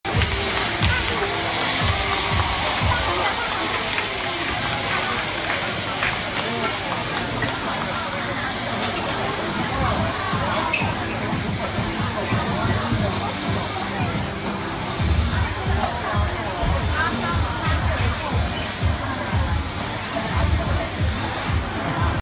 上海 星巴克 臻選烘焙工坊
上海 星巴克 臻選烘焙工坊「咖啡交響管及吧台儲豆管」五條名為「咖啡交響管」的銅管連結到咖啡大銅罐及各個吧台，借著氣壓讓烘好的咖啡豆在建築勿天花板之間飛翔，如雨點般進入五個儲豆罐裡。